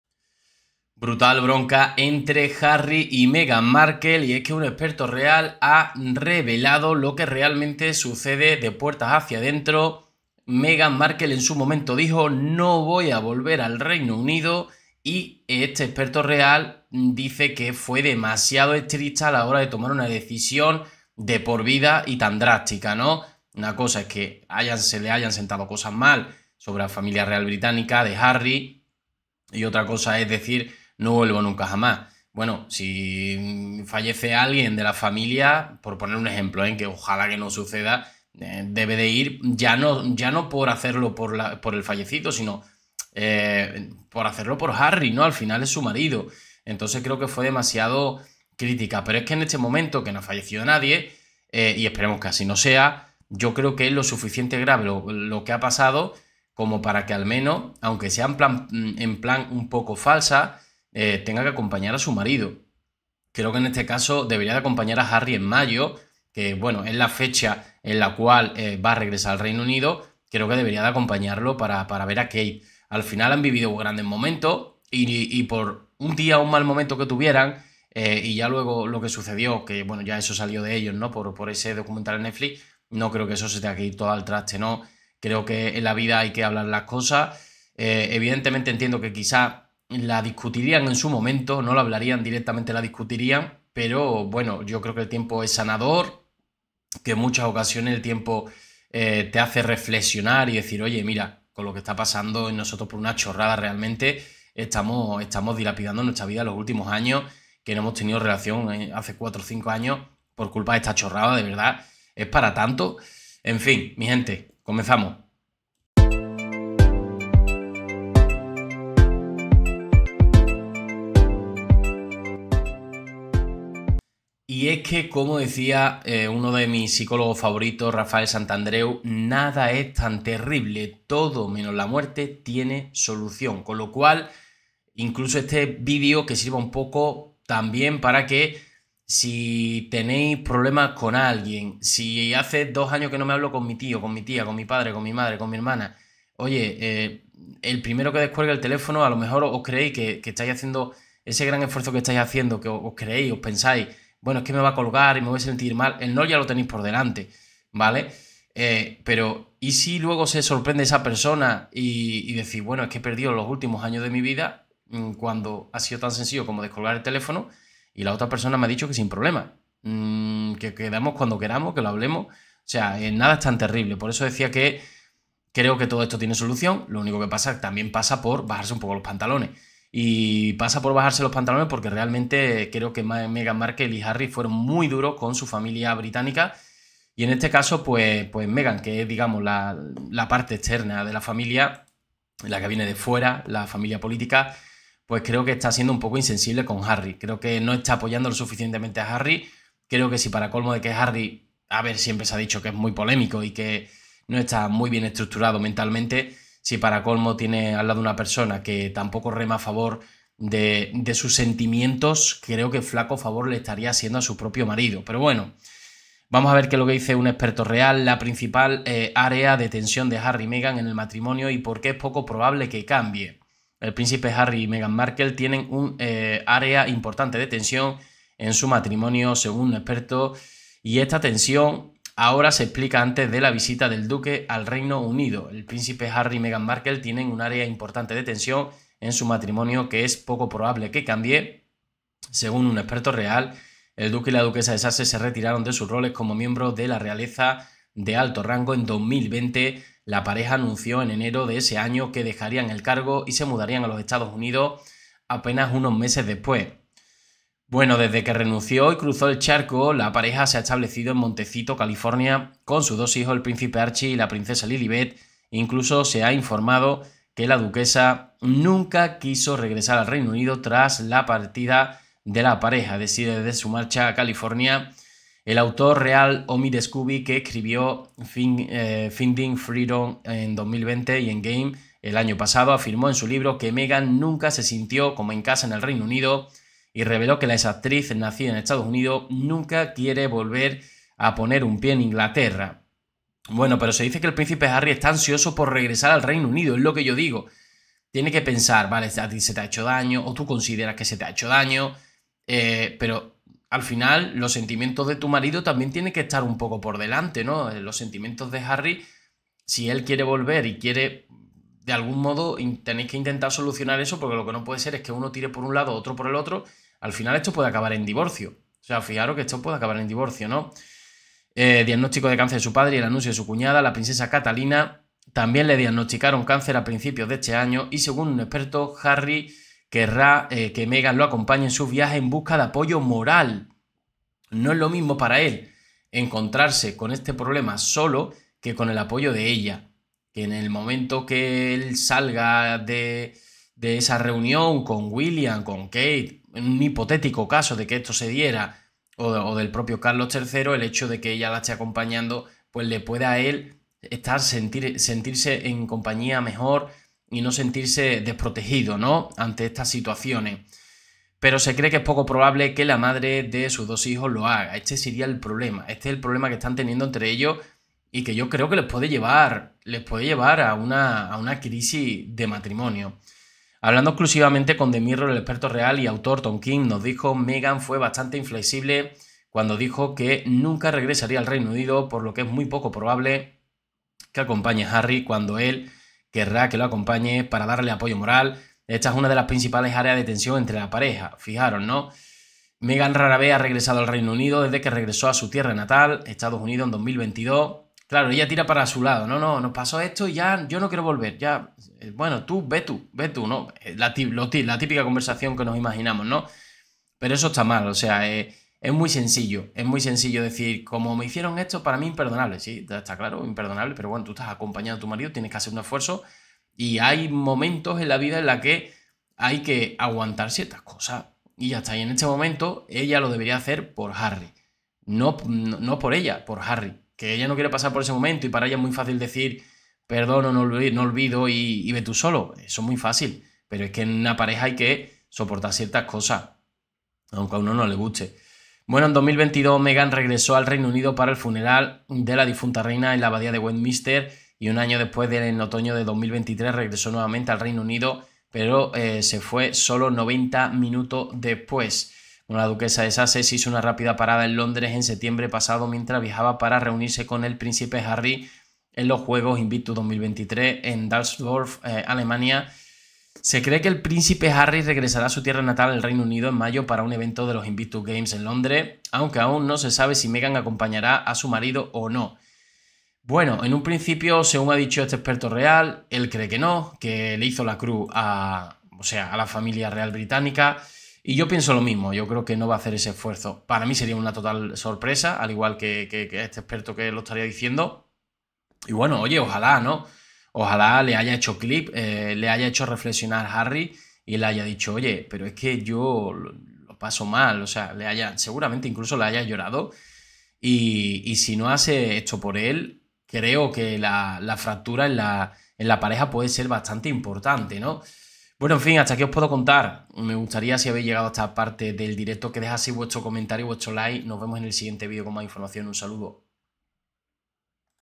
EXPERTO HABLA DE DIVORCIO BOMBA ENTRE HARRY Y MEGHAN MARKLE POR IMPEDIRLE VISITAR A KATE MIDDLETON
GRUPO WHATSAPP AVISOS DIRECTOS Y VÍDEOS👇
X @garcia_youtube
SI QUIERES SER INFLUENCER